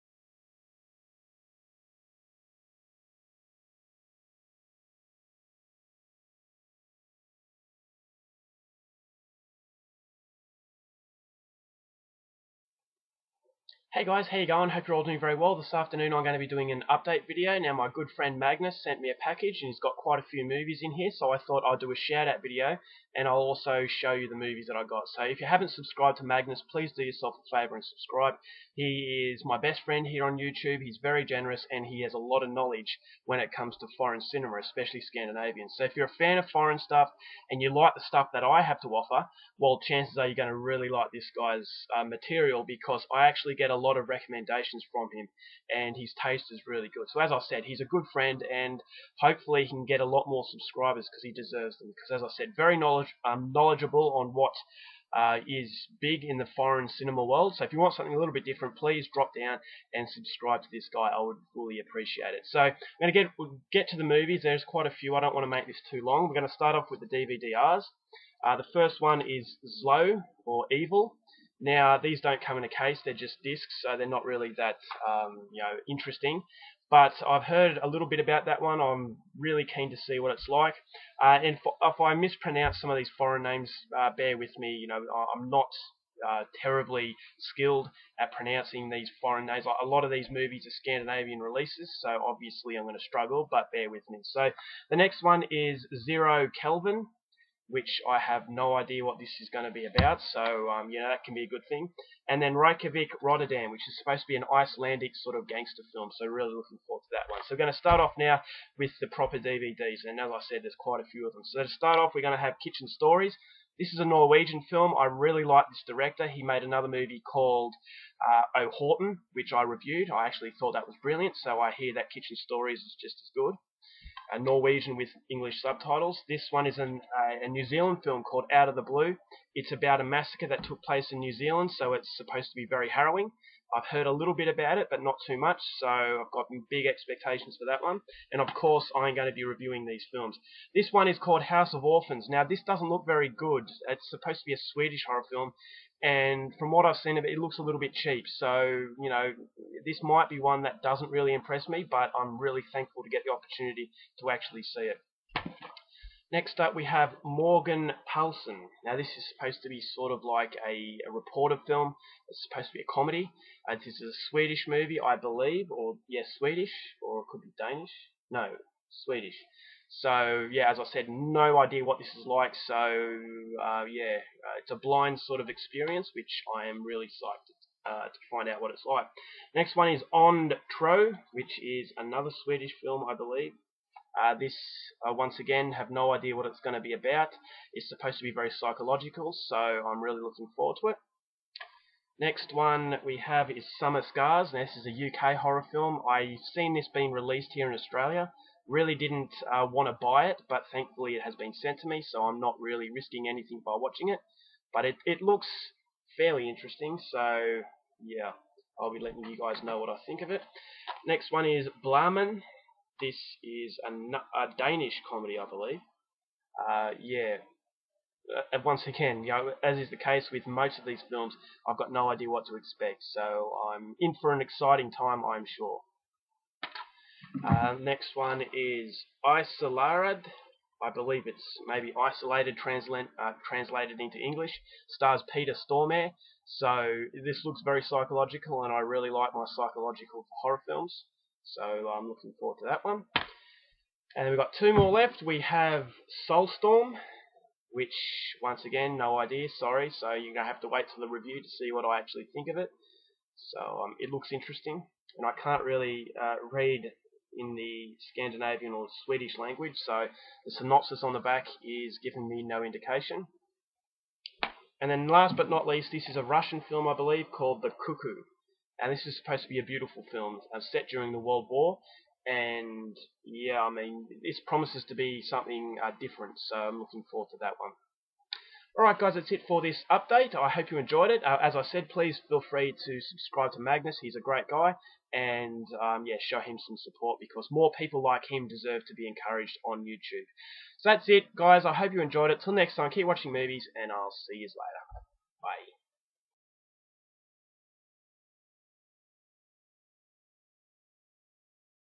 DVD Update + Shout Out
Showcasing some DVDs that were sent to me from my good friend Vaultkeeper2 aka Magnus. He is an extremely knowledgeable person when it comes to different films from across the world.